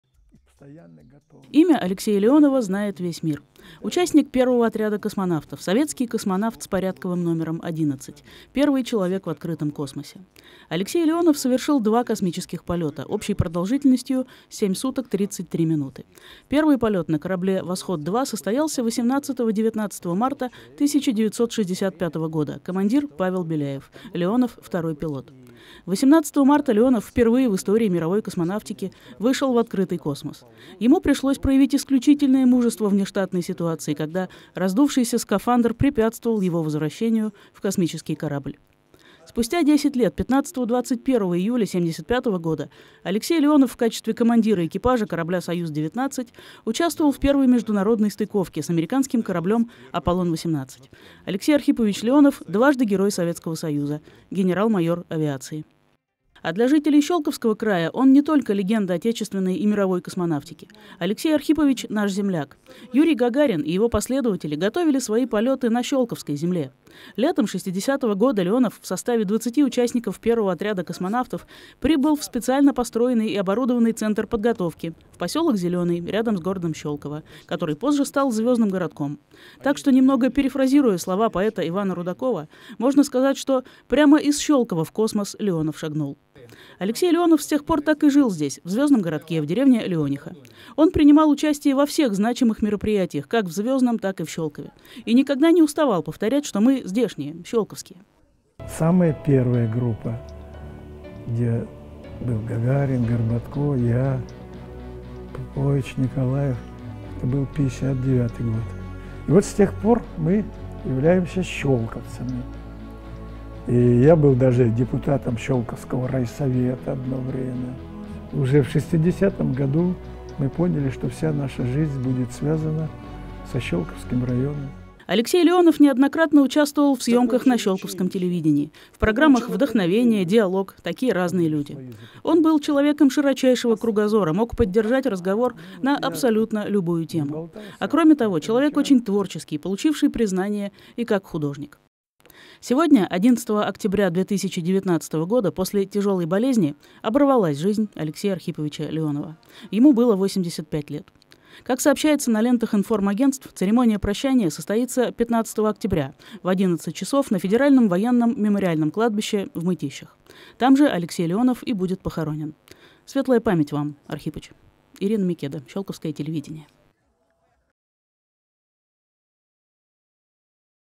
Вышел в вечность
На этой неделе не стало Алексея Леонова, легенды отечественной космонавтики, Дважды Героя Советского Союза, первого человека, покорившего открытый космос. Ему было 85 лет. Прощание состоится во вторник, 15 октября, на Федеральном военном мемориальном кладбище в Мытищах.